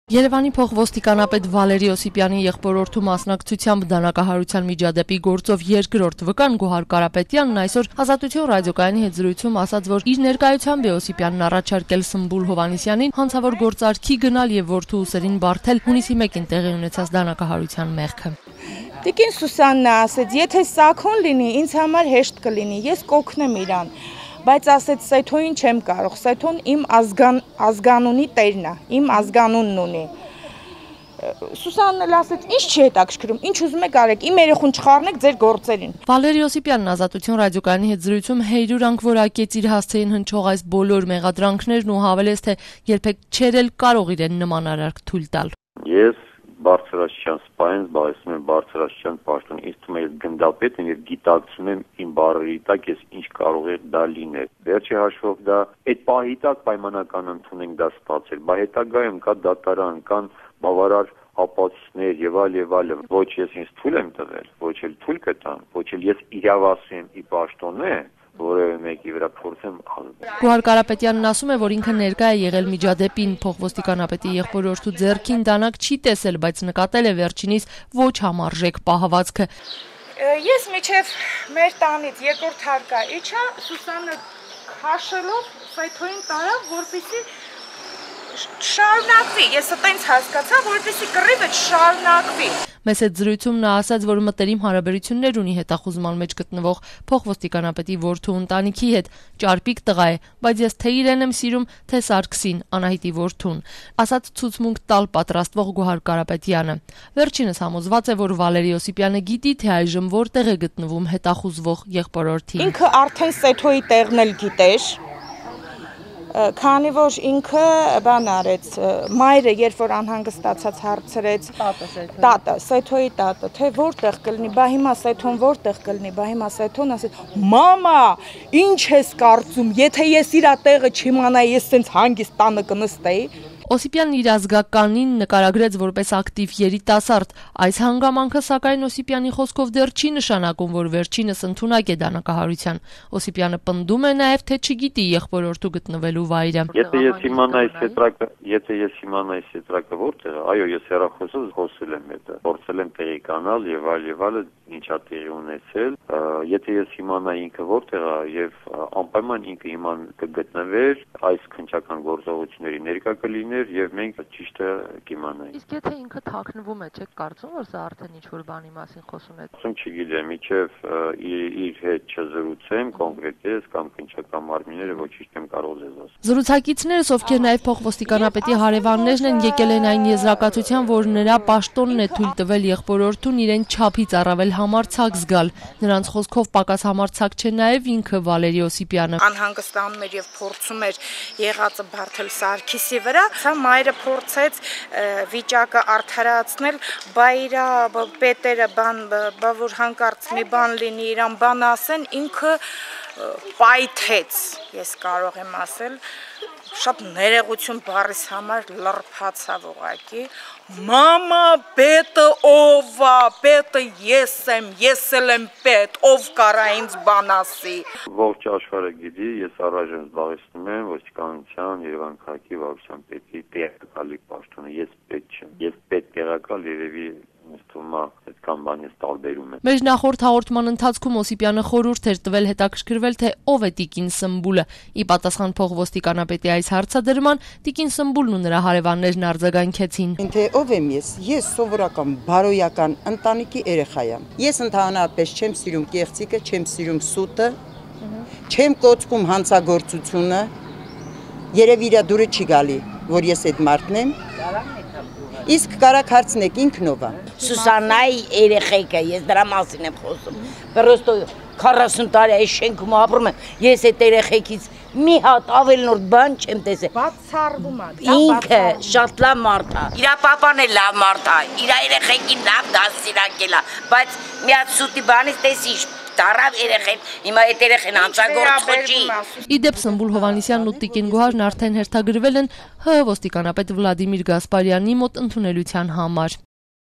Վկան պնդում է, թե Օսիպյանը իր ներկայությամբ է գործարք առաջարկել
Մայրաքաղաքի փոխոստիկանապետը, սակայն, կրկին կտրականապես հերքում է դա: